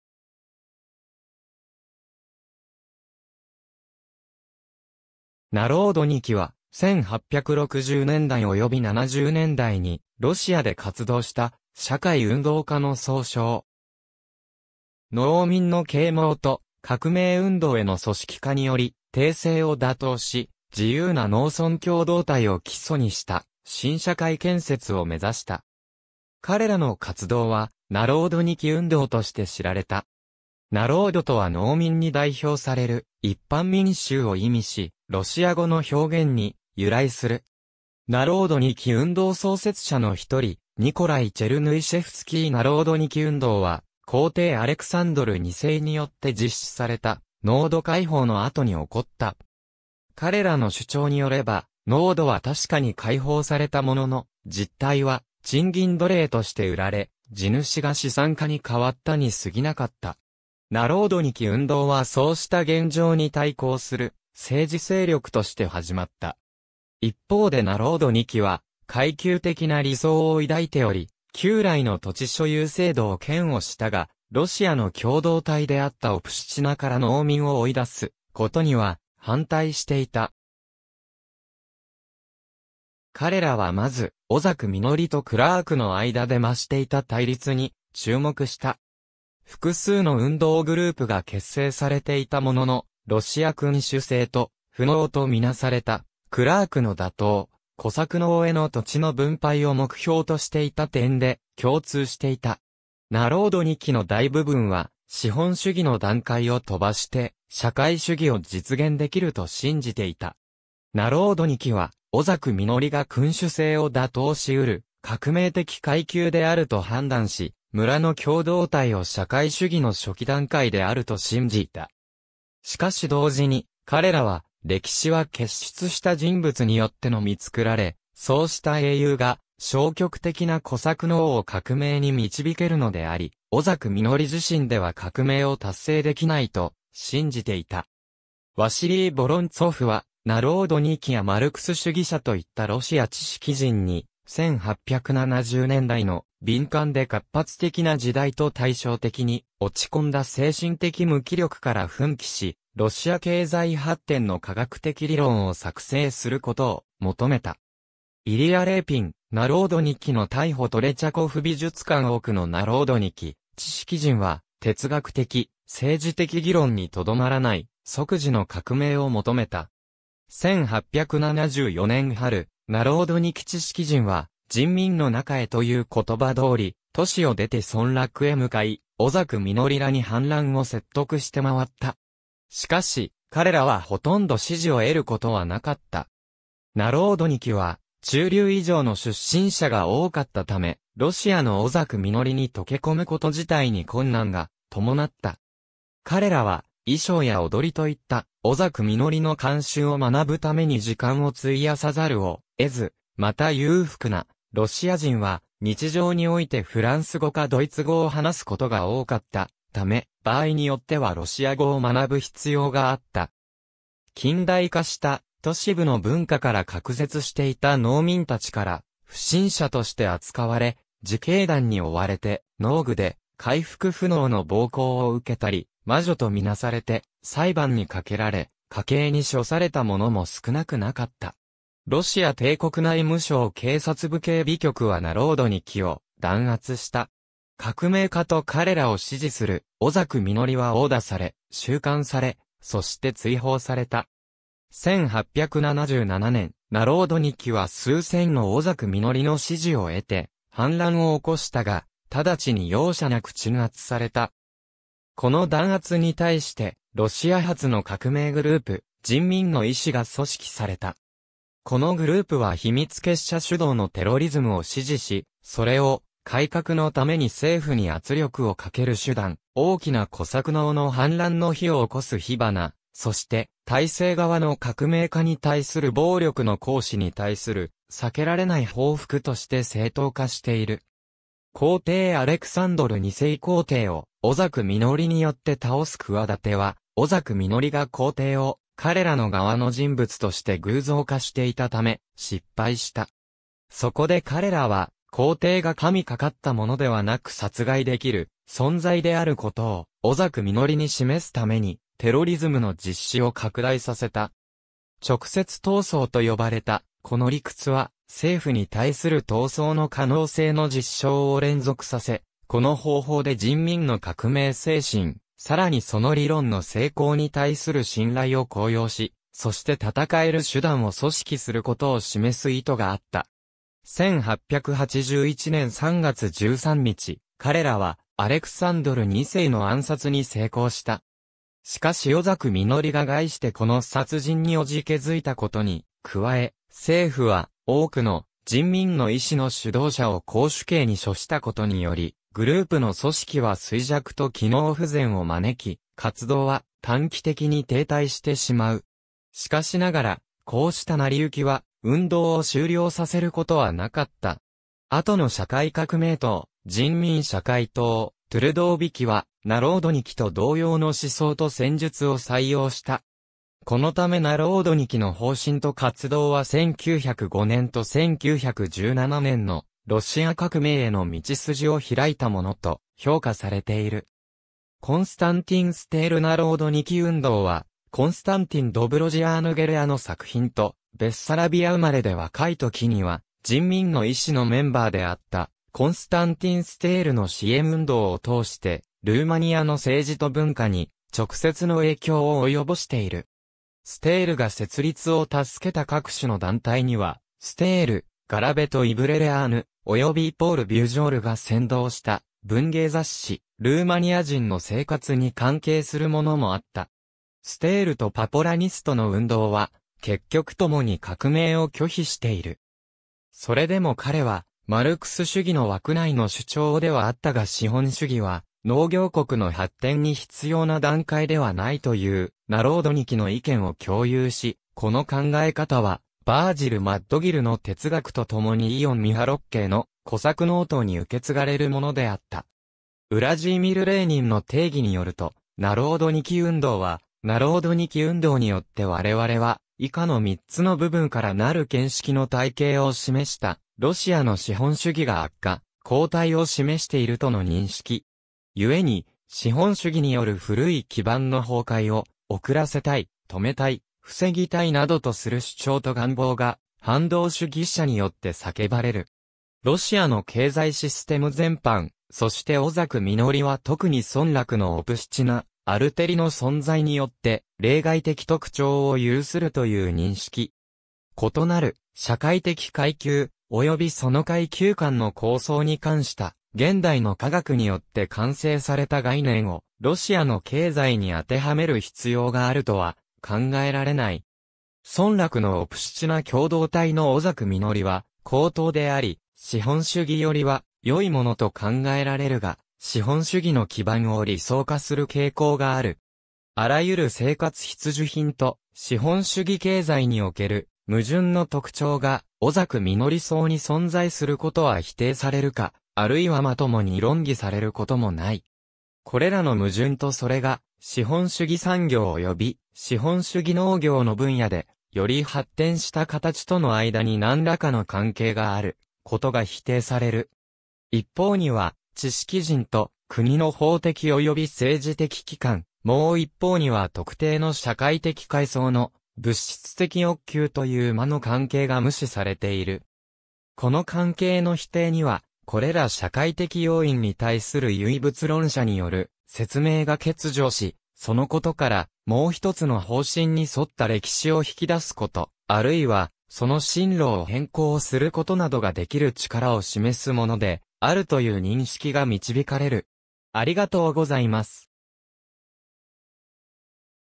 ナロードニキ
ロシア・ソ連の社会主義
ピョートル・ラヴロフ
ナロードニキ（露: народники）は、1860年代及び70年代にロシアで活動した社会運動家の総称。
農民の啓蒙と革命運動への組織化により帝政を打倒し、自由な農村共同体を基礎にした新社会建設を目指した。
彼らの活動はナロードニキ運動（ロシア語: народничество）として知られた。
ナロードとは農民に代表される一般民衆を意味し、ロシア語の表現 "Хождение в народ" (Hozhdenie v narod, 「人民のもとへ」)に由来する。
ナロードニキ運動創設者の一人ニコライ・チェルヌイシェフスキー ナロードニキ運動は、皇帝アレクサンドル2世によって実施された農奴解放（1861年）の後に起こった。
彼らの主張によれば、農奴は確かに解放されたものの、実態は賃金奴隷として売られ、地主が資産家に代わったに過ぎなかった。
ナロードニキ運動はそうした現状に対抗する政治勢力として始まった。
一方でナロードニキは懐旧的な理想を抱いており、旧来の土地所有制度を嫌悪したが、ロシアの共同体であったオプシチナ(Obshchina) から農民を追い出すことには反対していた。
彼らはまず、小作農とクラーク（自作農）の間で増していた対立に注目した。
複数の運動グループが結成されていたものの、ロシア君主制と「富農」とみなされたクラークの打倒・小作農への土地の分配を目標としていた点で共通していた。
ナロードニキの大部分は資本主義の段階をとばして社会主義を実現できると信じていた。
ナロードニキは小作農が君主制を打倒しうる革命的階級であると判断し、村の共同体を社会主義の初期段階であると信じた。
しかし同時に彼らは、歴史は傑出した人物によってのみ作られ、そうした英雄が消極的な小作農を革命に導けるのであり、小作農自身では革命を達成できないと信じていた(参照Great man theory)。
ワシリイ・ヴォロンツォフ(露: Василий Павлович Воронцов、英: Vasily Pavlovich Vorontsov)は、ナロードニキやマルクス主義者といったロシア知識人に「1870年代の敏感で活発的な時代と対照的に、落ち込んだ精神的無気力から奮起し、ロシア経済発展の科学的理論を作成」することを求めた。
イリヤ・レーピン 『ナロードニキの逮捕』（1880年～1889年, 1892年） トレチャコフ美術館 多くのナロードニキ知識人は、哲学的、政治的議論にとどまらない即時の革命を求めた。
1874年春、ナロードニキ知識人は「人民の中へ（ヴ・ナロード）」という言葉通り、都市を出て村落へ向かい、小作農らに反乱を説得して回った。
しかし、彼らはほとんど支持を得ることはなかった。
ナロードニキは中流以上の出身者が多かったため、ロシアの小作農に溶けこむこと自体に困難が伴った。
彼らは衣装や踊りといった小作農の慣習を学ぶために時間を費やさざるを得ず、また裕福なロシア人は日常においてフランス語かドイツ語を話すことが多かったため、場合によってはロシア語を学ぶ必要があった。
近代化した都市部の文化から隔絶していた農民たちから不審者として扱われ、自警団に追われて農具で回復不能の暴行を受けたり、魔女と見なされて裁判にかけられ火刑に処された者も少なくなかった。
ロシア帝国内務省警察部警備局はナロードニキを弾圧した。
革命家と彼らを支持する小作農は殴打され、収監され、そして追放された。
1877年、ナロードニキは数千の小作農の支持を得て反乱を起こしたが、ただちに容赦なく鎮圧された。
この弾圧に対して、ロシア初の革命グループ「人民の意志」が組織された。
このグループは秘密結社主導のテロリズムを支持し、それを「改革のために政府に圧力をかける手段、大きな小作農の反乱の火を起こす火花、そして体制側の革命家に対する暴力の行使に対する避けられない報復」として正当化している。
皇帝アレクサンドル2世 皇帝を小作農によって倒す企ては、小作農が皇帝を「彼らの側の」人物として偶像化していたため失敗した。
そこで彼らは、皇帝が神懸ったものではなく殺害できる存在であることを小作農に示すために、テロリズムの実施を拡大させた。
「直接闘争」と呼ばれたこの理屈は「政府に対する闘争の可能性の実証を連続させ、この方法で人民の革命精神、さらにその理...